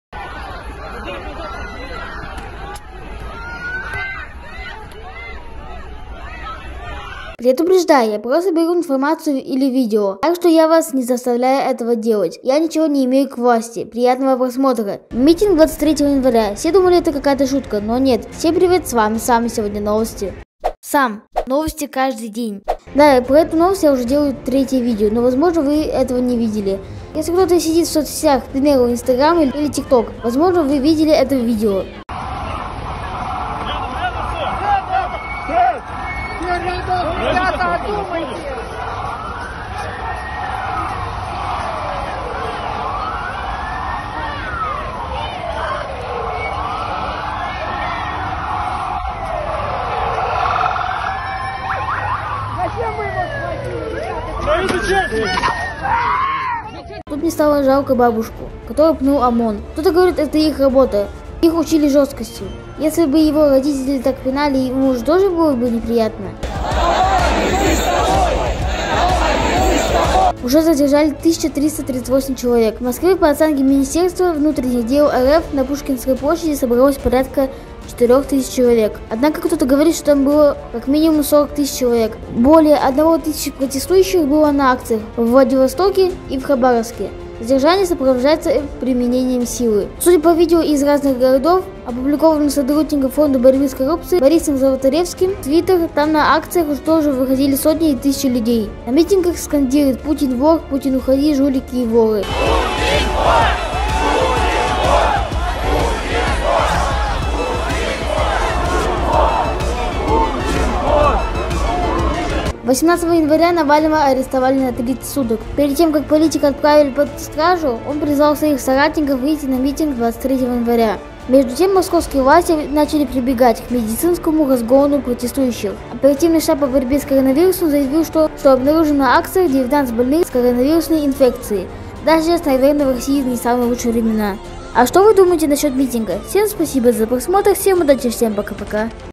БАБУШКУ ПНУЛИ ОМОНЫ! | ЧТО СЛУЧИЛОСЬ С ЛЮДЬМИ? | МИТИНГ 23 ЯНВАРЯ | АЛЕКСЕЙ НАВАЛЬНЫЙ
Всем привет С вами САМ!И сегодня НОВСТИ!